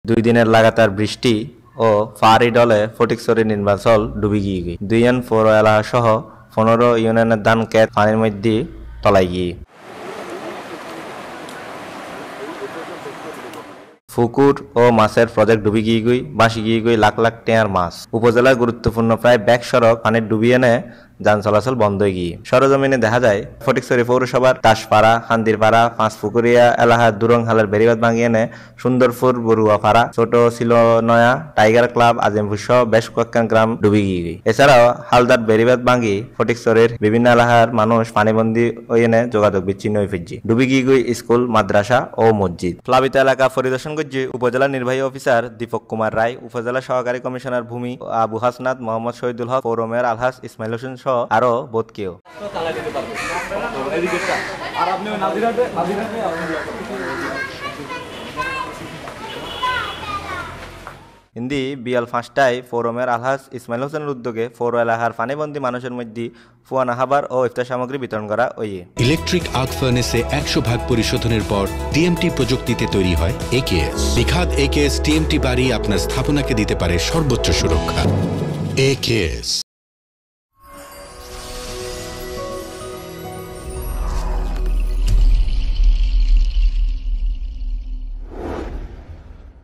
ফটিকছড়িতে ভারি বর্ষন ও খালের ভাঙ্গনে লক্ষাধিক মানুষ পানিবন্দী : যোগাযোগ বিচ্ছিন্ন!
দুই দিনের টানা ভারী বর্ষন ও উজান থেকে নেমে আসা পাহাড়ী ঢলে ফটিকছড়ির নিন্মাঞ্চল প্লাবিত হয়েছে। ২ টি পৌর এলাকাসহ ১৫ টি ইউনিয়নের শস্য ক্ষেত্র ও রোপা আমন বন্যার পানিতে তলিয়ে গেছে। পুকুর ও মাছের প্রজেক্ট ডুবে, ভেসে গেছে লক্ষ লক্ষ টাকার মাছ। উপজেলার গুরুত্বপুর্ন প্রায় সড়ক পানিতে ডুবে যাওয়ায় যান চলাচল ব্যাহত হচ্ছে।সরেজমিনে গিয়ে দেখা যায়, ফটিকছড়ি পৌরসভাধীন দাশ পাড়া, কান্দিরপাড়া, পাঁচ পুকুরিয়া এলাকায় ধুরুং খালের বেড়িবাধ ভেঙ্গে সুন্দরপুর বড়ুয়া পাড়া, ছোটছিলোনয়া, টাইগার ক্লাব, আজিমপুরসহ বেশ কয়েকটি গ্রাম প্লাবিত হয়। এছাড়াও হালদার বেড়ীবাধ ভেঙ্গে ফটিকছড়ি হেয়াঁকো সড়কের বেশ কয়েকটি স্থানে ডুবে যাওয়ায় দাঁতমারা, নারায়ানহাট, ভূজপুরে যোগাযোগ বিচ্ছিন্ন হয়ে পানিবন্দী হয়ে পড়েছে এলাকার মানুষ। ডুবে গেছে স্কুল, মাদ্রাসা ও মসজিদ।এদিকে কাজিরহাট নাজিরহাট এলজিইডি সড়কে হারুয়ালছড়ি ইউপির কাজী বাড়ি এলাকায় ফটিকছড়ি খালের বেড়ীবাধ ভেঙ্গে যাওয়ায় সড়কটিতে যান চলাচল বন্ধ রয়েছে। অপরদিকে দক্ষিন ফটিকছড়ির নানুপুর, ধর্মপুর, বক্তপুর, আব্দুল্লাপুরের প্রায় এলাকা পানিতে ভাসছে।
প্লাবিত এলাকা পরিদর্শন করেন উপজেলা নির্বাহী অফিসার দীপক কুমার রায়, উপজেলা সহকারি কমিশনার ভূমি আবু হাসনাত মো: শহিদুল হক, পৌর মেয়র আলহাজ্ব ইসমাইল হোসেন, চেয়ারম্যান  শাহনেওয়াজ, কমিশনার গোলাপ মওলা গোলাপ, বিবিরহাট বনিক কল্যান সমিতির সভাপতি দিদারুল বশর চৌধুরী দুদু ও সম্পাদক দুলাল। এদিকে বিকাল ৫ টায় পৌর মেয়র আলহাজ্ব ইসমাইল হোসেনের উদ্যোগে পৌর এলাকার পানিবন্দী মানুষদের মাঝে শুকনা খাবার ও ইফতার সামগ্রী বিতরন করেন।